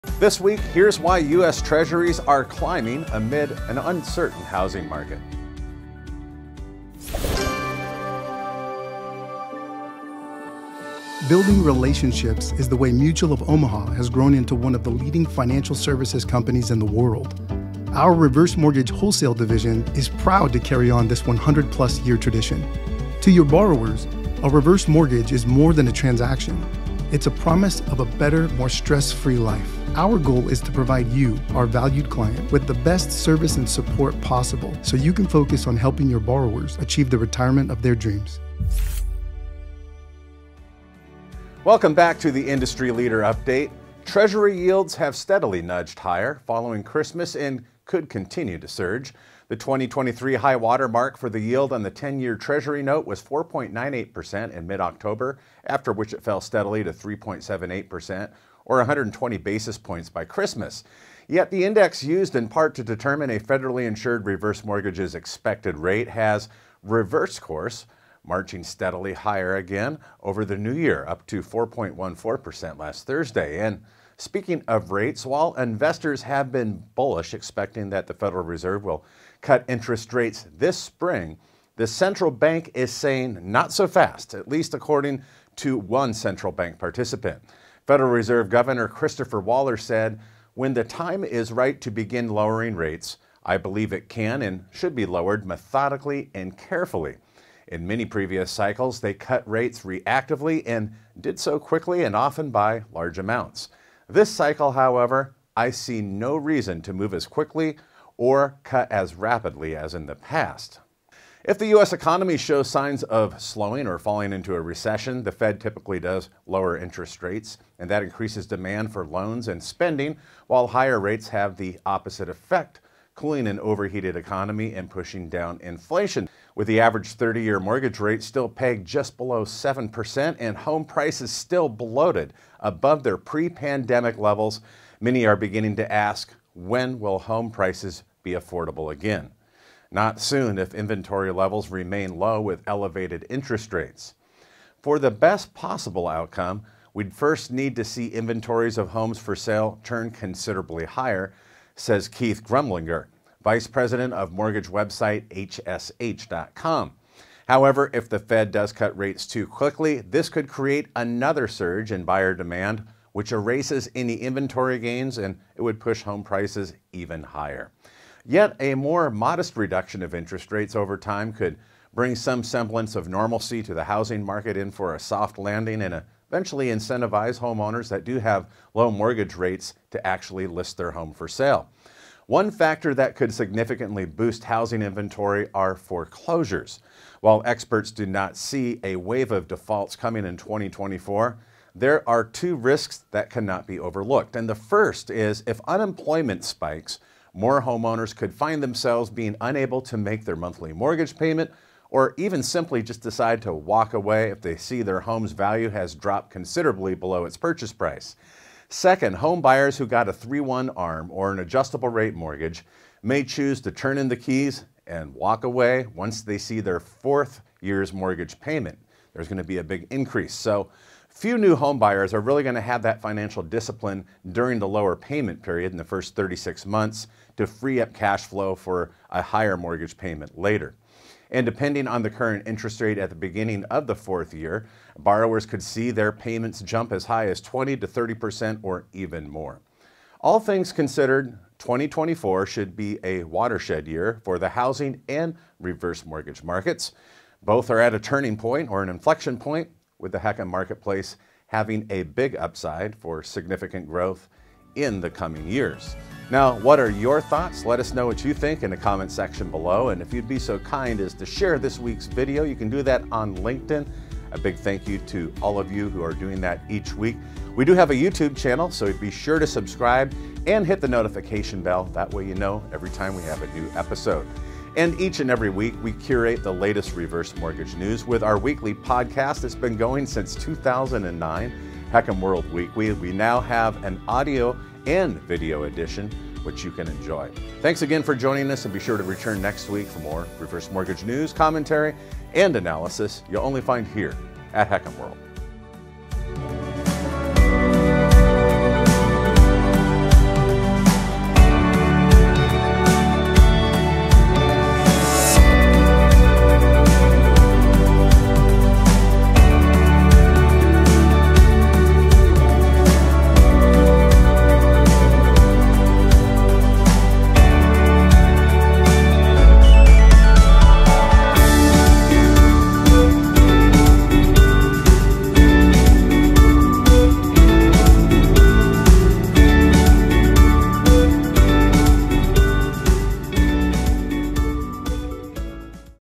Treasuries Rates Climb While the Housing Market Remains Uncertain: The Industry Leader Update
Treasury yields have steadily nudged higher following Christmas and could continue to surge. The 2023 high-water mark for the yield on the 10-year Treasury note was 4.98% in mid-October after which it fell steadily to 3.78% or a 120 basis points by Christmas. Yet the index used in part to determine a federally-insured reverse mortgage’s expected rate has reversed course marching steadily higher over the new year up to 4.14% last Thursday.

And speaking of rates while investors has been bullish expecting that the Fed will cut interest rates this spring, the central bank is saying not so fast, at least according central bank participant. Federal Reserve Governor Christopher Waller said, “When the time is right to begin lowering rates, I believe it can and should be lowered methodically and carefully. In many previous cycles ... they cut rates reactively and did so quickly and often by large amounts. This cycle, however, ... I see no reason to move as quickly or cut as rapidly as in the past.”

If the U.S. economy show signs of slowing or falling into a recession the Fed typically lowers interest rates increases demand for loans and spending while higher rates have the opposite effect cooling an overheated economy or high inflation.

With the average 30-year mortgage rate still pegged just below seven percent and home prices still bloated above their pre-pandemic levels many are beginning to ask- when will home prices be affordable again? Not soon if inventory levels remain low with elevated interest rates. “For the best possible outcome, we’d first need to see inventories of homes for sale turn considerably higher”, says Keith Gumbinger, vice president of mortgage website HSH.com. However, if the Fed cuts rates too quickly it could create another surge in buyer demand that erases any inventory gains and pushing home list prices even higher. Yet a more modest reduction of rates over time could bring some semblance of normalcy to the housing market and eventually incentivize homeowners with low mortgage rates to sell.

One factor that could significantly boost housing inventory is foreclosures. While experts don’t expect to see a wave of defaults in 2024 there are two risks that cannot be overlooked. First, if unemployment spikes more homeowners could find themselves unable to make their mortgage payment or even decide to walk away if their home’s value has dropped considerably. Second, homebuyers who got a 3/1 ARM or adjustable rate mortgage may choose to turn in the keys and walk once they see their fourth-year mortgage payment. Few new homebuyers will have the financial discipline to retire debt during the first 36 months of the loan freeing up cashflow for a higher mortgage payment. Depending on the current interest rate at the beginning of the fourth year borrowers could see their payments jump 20-30% or more.

All things considered 2024 should be watershed year for the housing  and reverse mortgage markets. Both are at a turning point, with the HECM marketplace having a big upside for significant growth in the coming years.